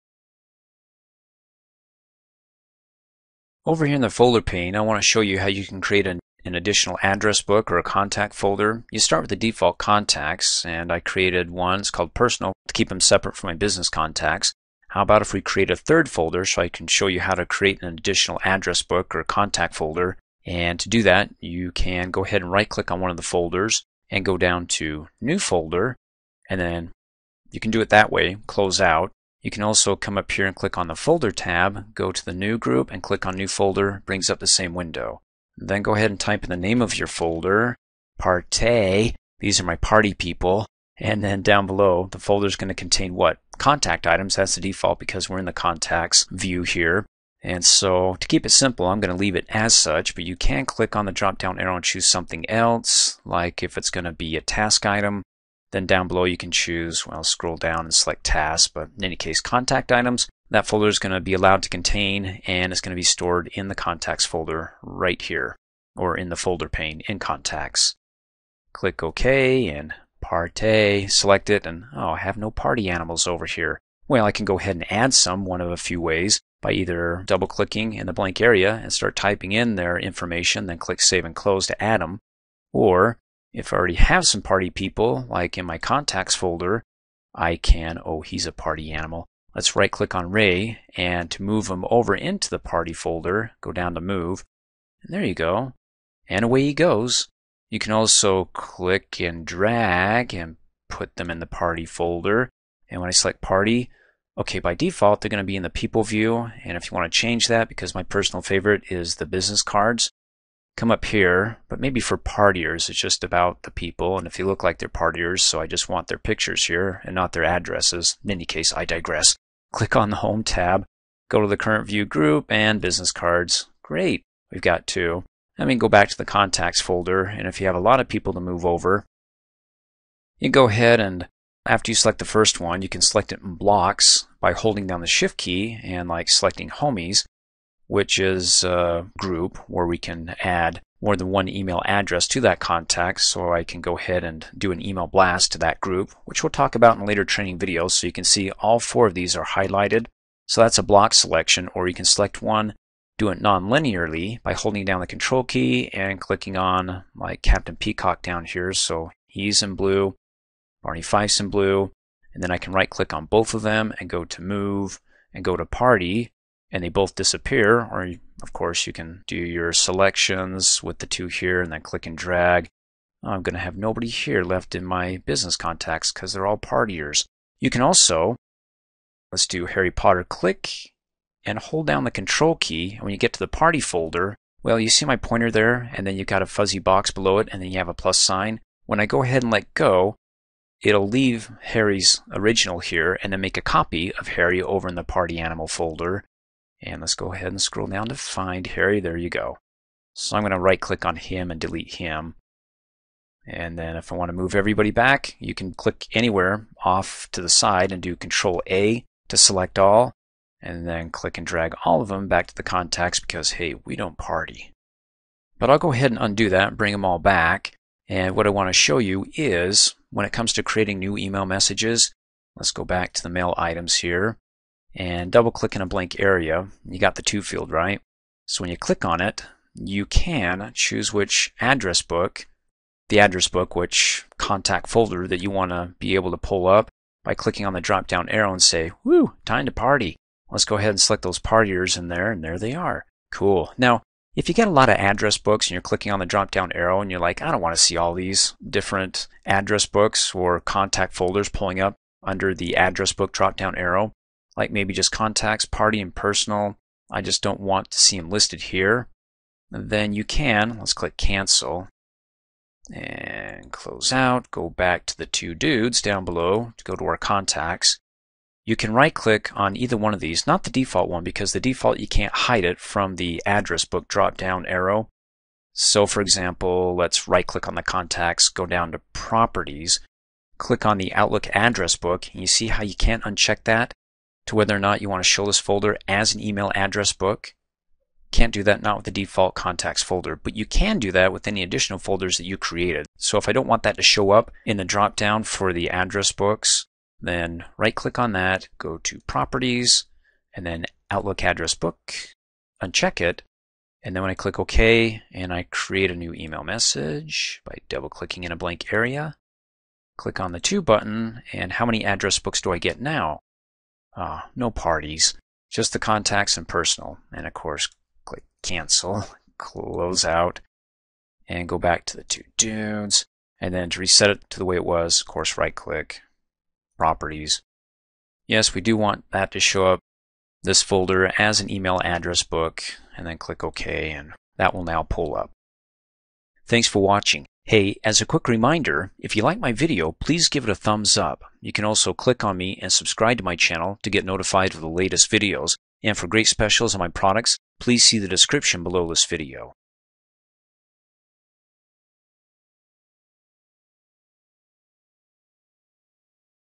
Microsoft Outlook 2016: Create Additional Address Books
Microsoft Outlook 2016 training video on how to create additional address books for contact folders.